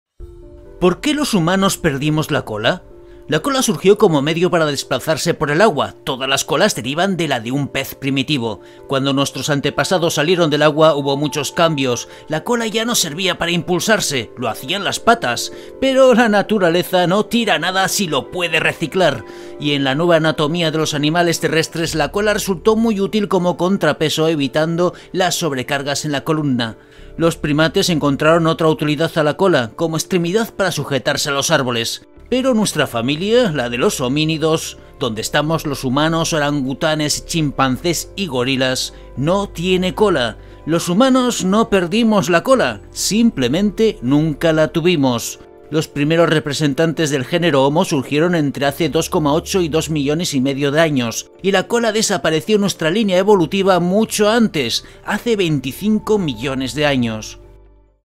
¿POR QUÉ los HUMANOS PERDIMOS la COLA? La respuesta puede que te sorprenda
La cola surgió como medio para desplazarse por el agua, todas las colas derivan de la de un primitivo pez. Cuando nuestros antepasados salieron del agua hubo muchos cambios la cola ya no servía para impulsarse, lo hacían las patas, pero la Naturaleza no tira nada si lo puede reciclar; y en la nueva anatomía de los animales terrestres la cola resultó útil como contrapeso evitando las sobrecargas en la columna. Los primates encontraron otra utilidad a la cola como extremidad para sujetarse a los árboles. Pero nuestra familia, la de los homínidos, donde estamos los humanos, orangutanes, chimpancés y gorilas, no tiene cola. Los humanos no perdimos la cola simplemente nunca la tuvimos, los primeros representantes del género homo surgieron hace entre 2,8 y 2,5 millones de años y la cola desapareció en nuestra línea evolutiva mucho antes hace unos 25 millones de años.

EXOPLANETAS:
Ayuda a crear más y mejores vídeos: